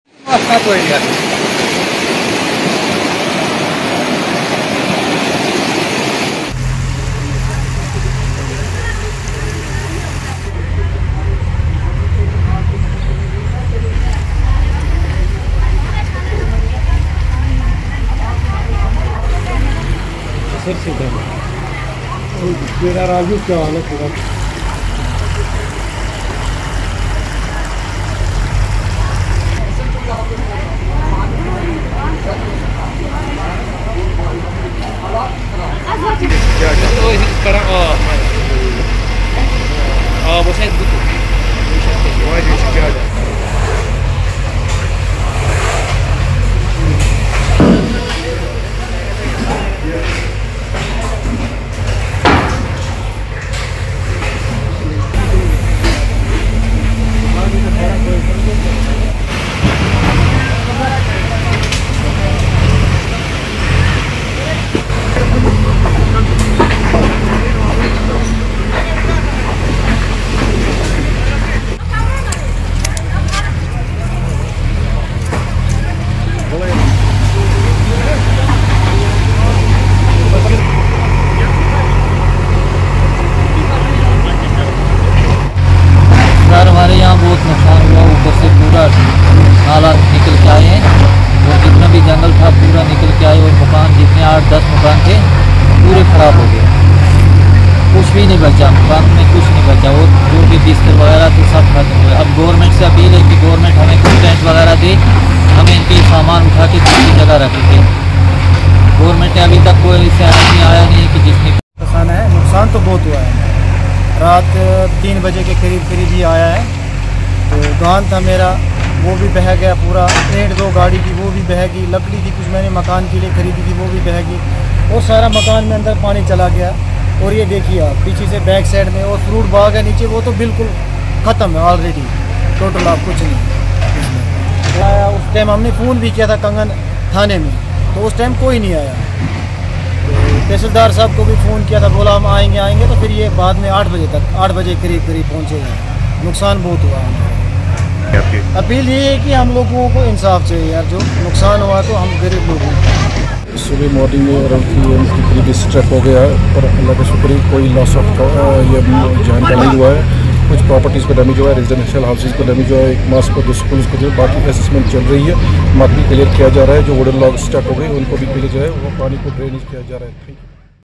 Several houses, Mosque damaged as cloudburst hits Ganderbal village.
Srinagar, July 29 : Several residential houses, school and a Mosque were damaged after cloudburst hit Gujar Pati Prang in central Kashmir’s Ganderbal district in the wee hours on Saturday.

An official told that following heavy rainfall in the area, cloudburst hit Gujar Pati Prang.

He said that in the in the incident several residential houses, a government school and a Mosque were damaged.

He said that SDM and Tehsildar Kangan, along with officials have reached to the spot to review the situation.